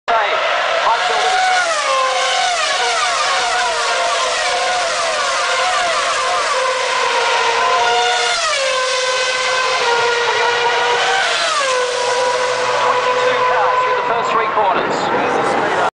Australian F1 GP '07 - Start of Race
All 22 cars heading up to Turn 3 for the first time in the race.